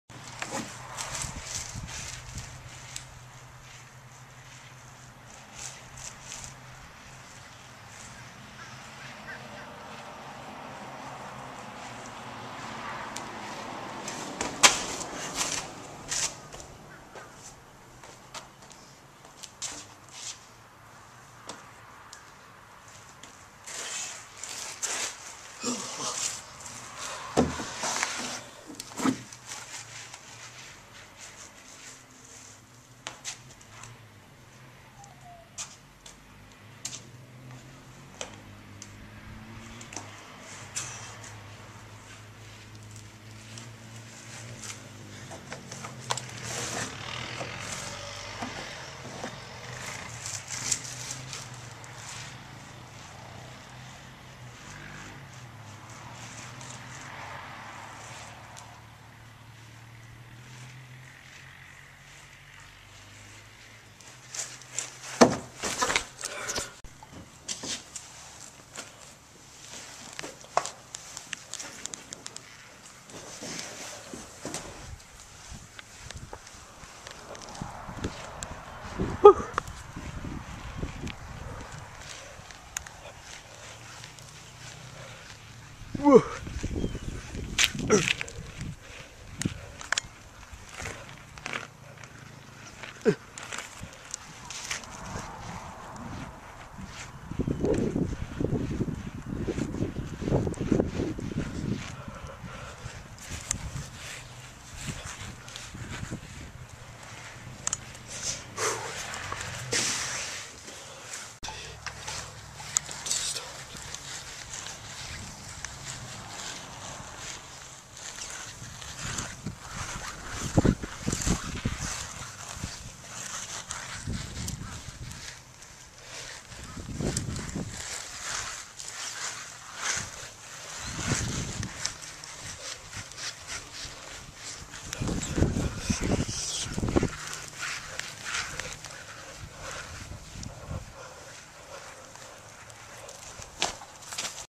Unicycle
I was in the unicycle club in 8th grade, I was one of the best ones because I was the only one who rarely fell plus, the others were kinda fat and unbalanced :P